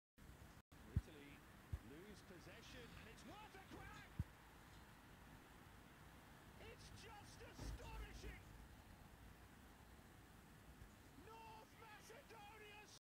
Trajkovski scores last minute Extraordinary Goal 🔥 Italy out and misses the Qatar World Cup
Italy out of world Cup
Travkovski Macedonia god
Macedonia goal vs Italy